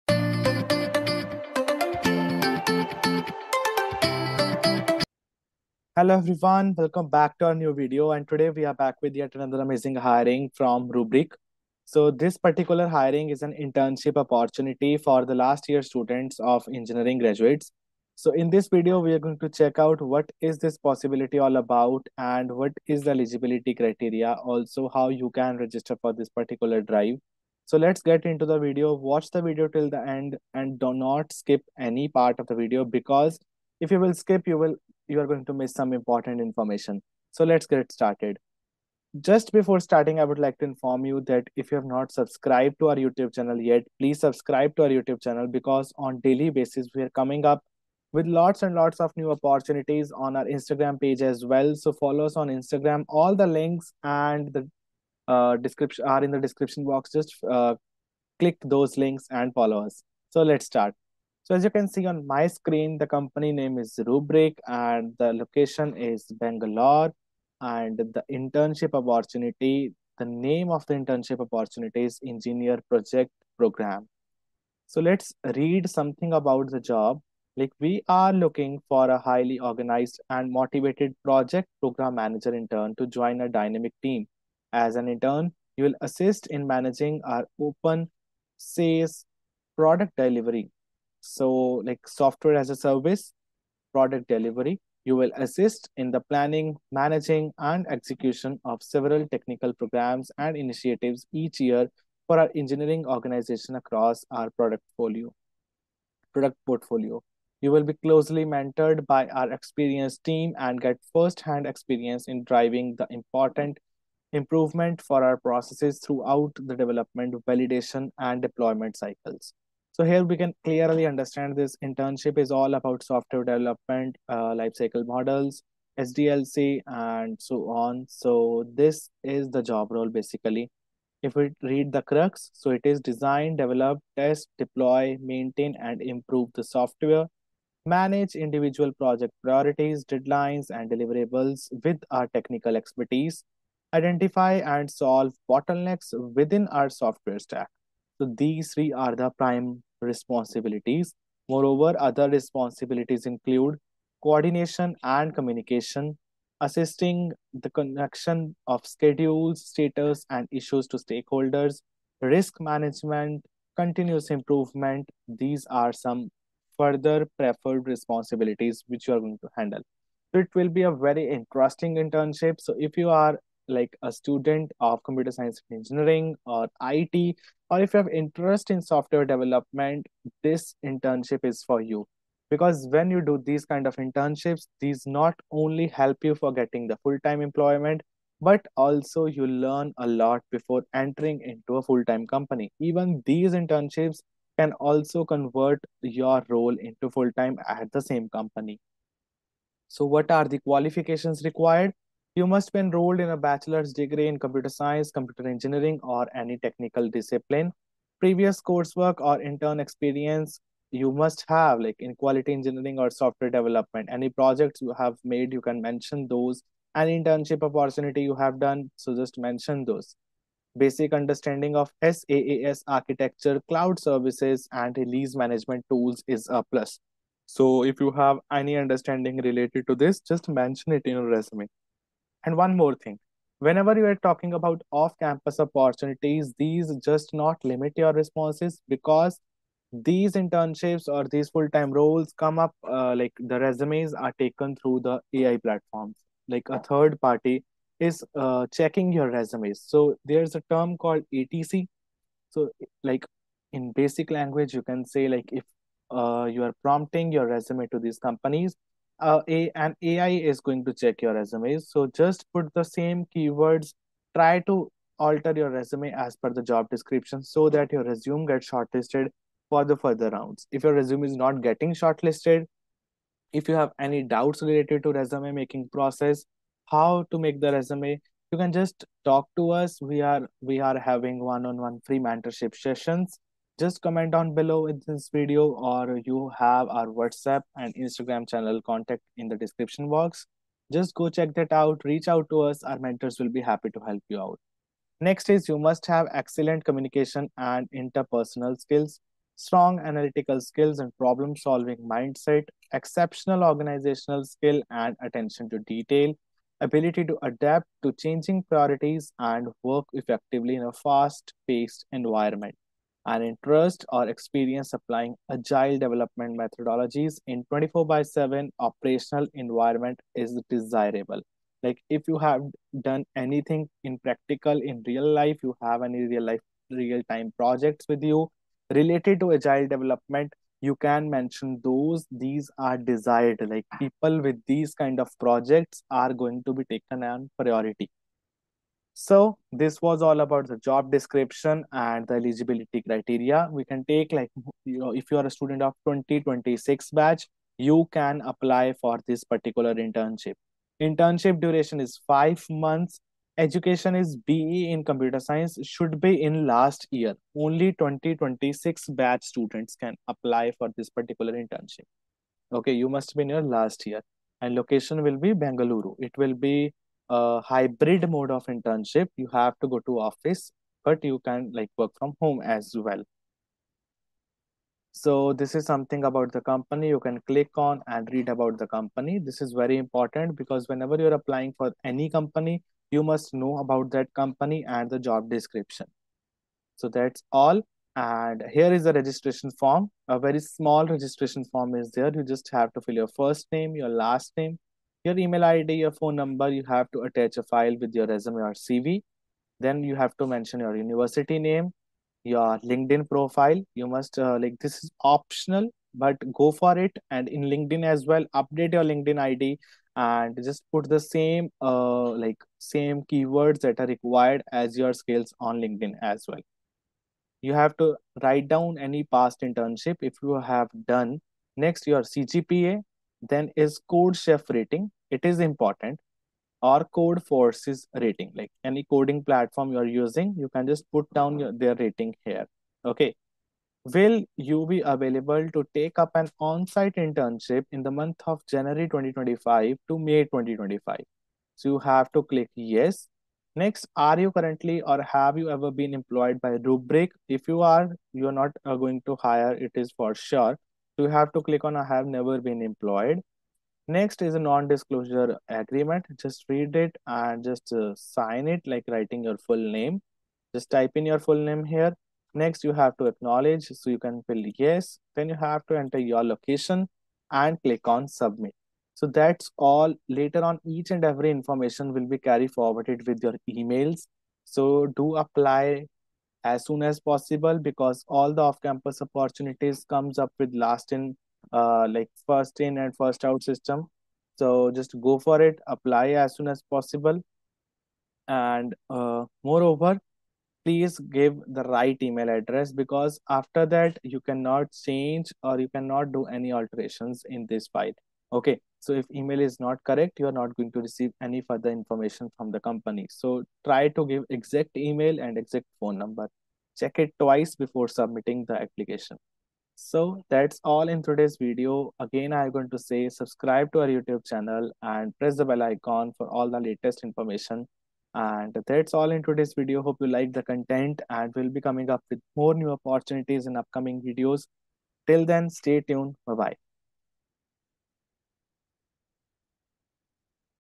🚀 Rubrik is Hiring ! 2026 Batch Interns | Apply Now!
🔥 Exciting Internship Opportunity at Rubrik! 🚀

Rubrik is hiring Project/Program Manager Interns for a 5-month onsite internship in Bengaluru from January 2025 to May 2025. If you're a final-year BE Computer Science student, this is your chance to work on on-prem & SaaS product delivery, risk management, and agile development methodologies!

📌 Internship Details:
✅ Role: Project/Program Manager Intern
✅ Location: Bengaluru (Onsite)
✅ Eligibility: BE in Computer Science (Final Year)
✅ Duration: 5 Months (Jan 2025 – May 2025)
✅ Skills: Project management, SaaS architecture, Agile methodologies

💡 Gain hands-on experience in software development processes, risk management, and process improvement while working with experienced mentors!

💡 Looking for Placement Preparation? Get expert guidance from Talent Battle, India’s leading placement preparation platform! 🎯